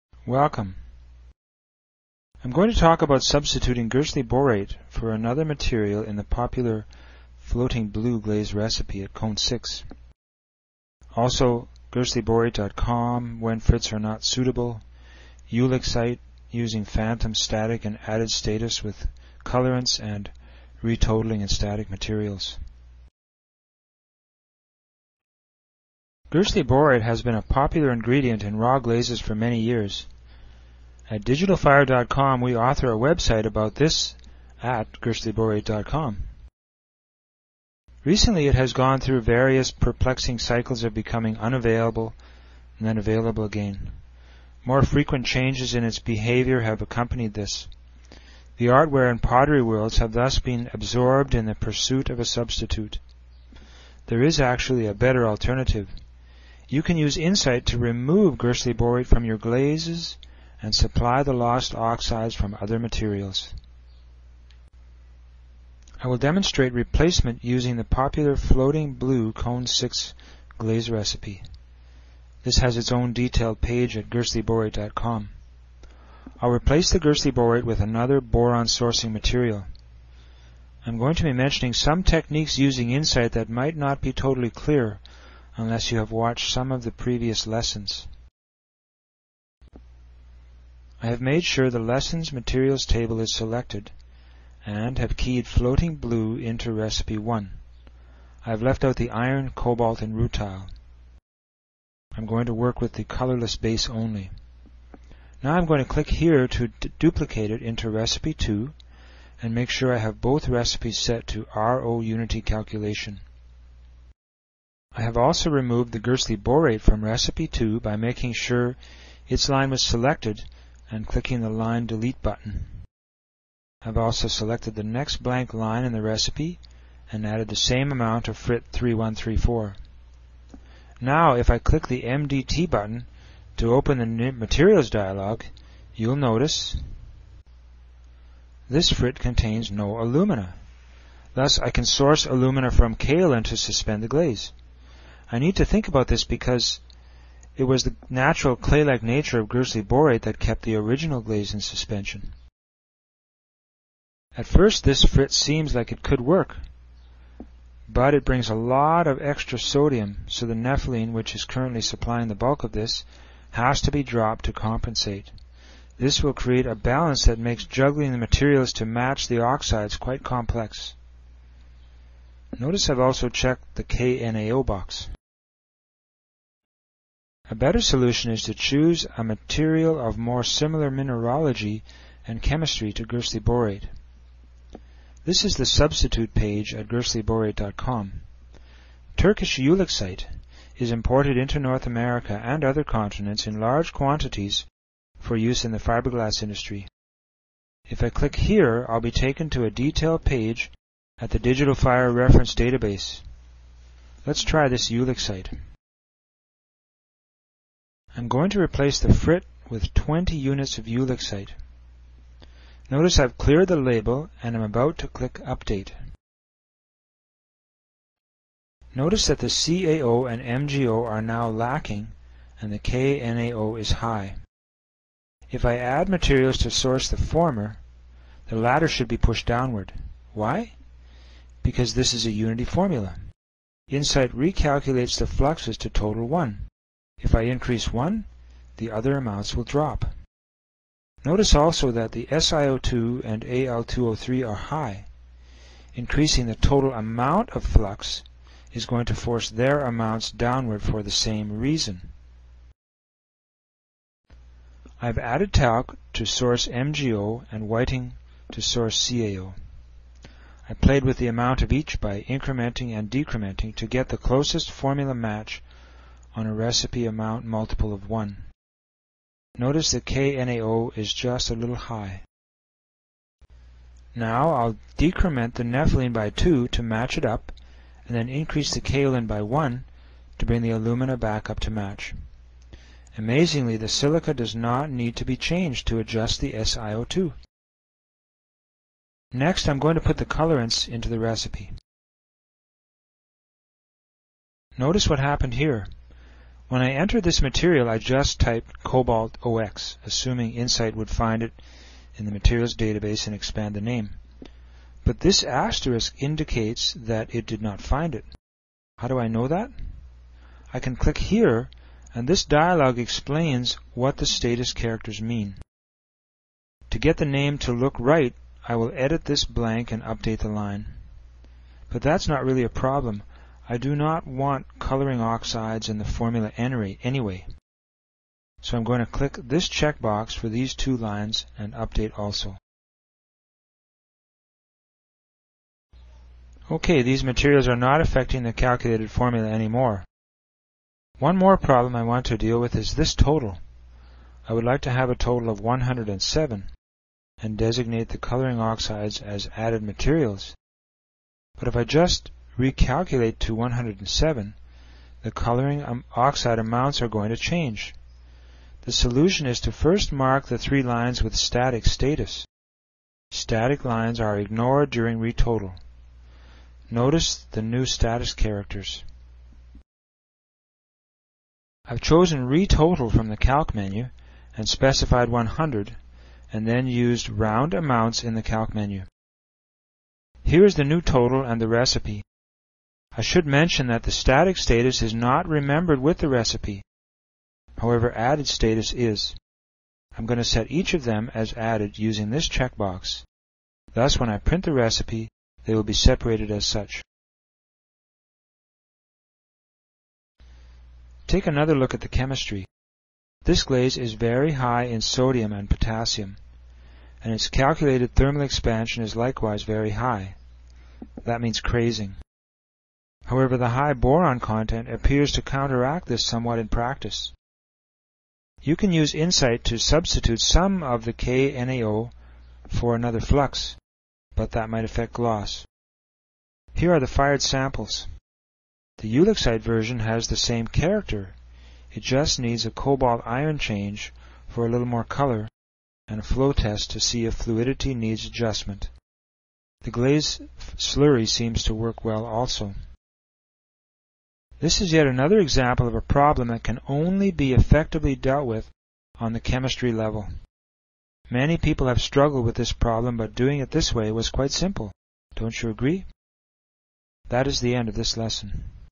Substituting Gerstley Borate in Floating Blue
Using Digitalfire INSIGHT to formulate, fix and adjust ceramic glazes.
Gerstleyborate.com, when frits are not suitable, ulexite, using phantom, static and added status with colorants and retotaling and static materials.